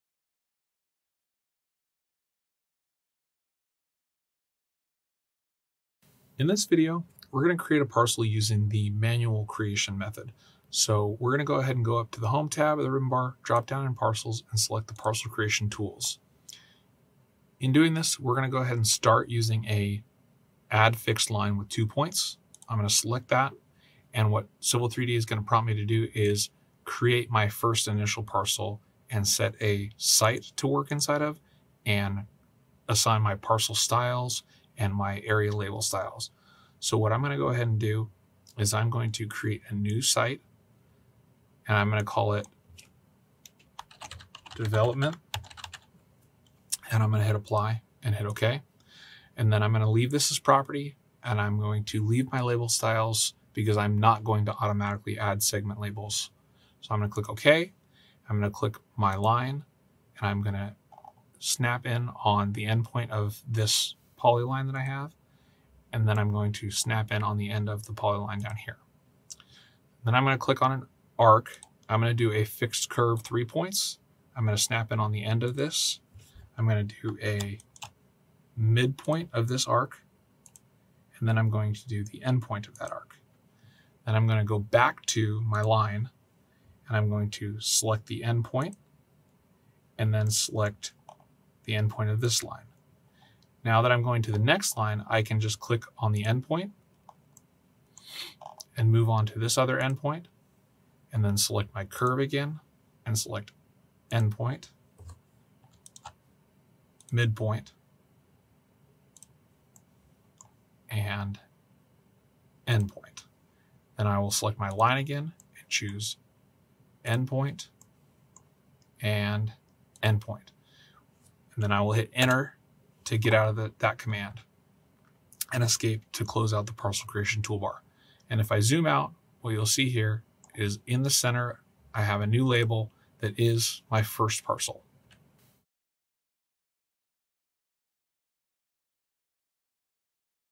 Cert Prep for Autodesk Pro Civil 3D Infrastructure Design Lesson 1: Create parcel by layout
After completing this lesson, you'll be able to:

Create a parcel using the manual creation method

Objective Domains Covered:

1.4.b Create a parcel by layout